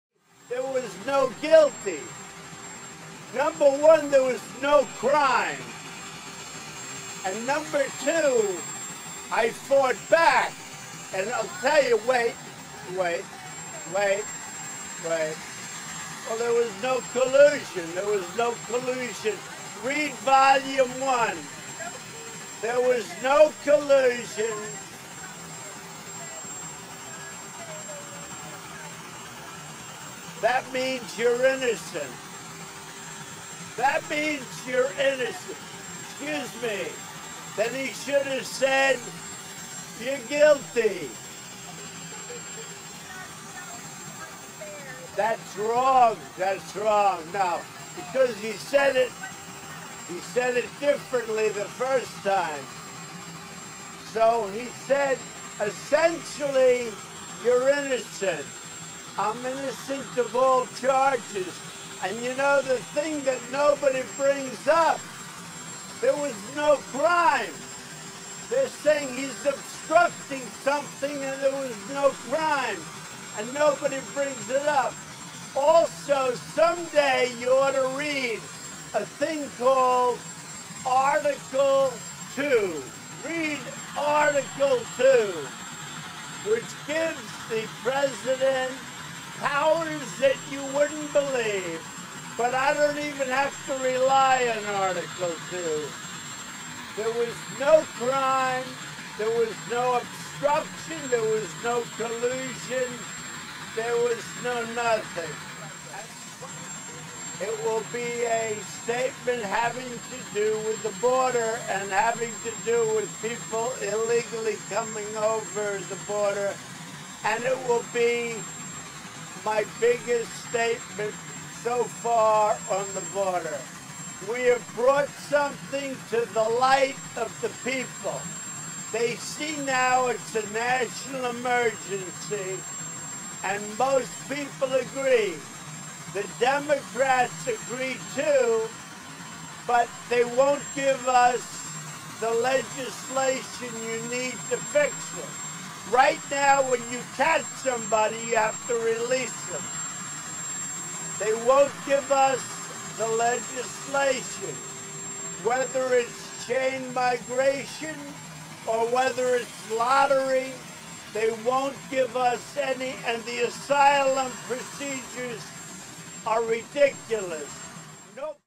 Is Trump Drunk?
No. He is not drunk. But it's quite funny.
This is a simple video trick to demonstrate how easy it is to make someone appear drunk. Similar to what someone did to Nancy Pelosi.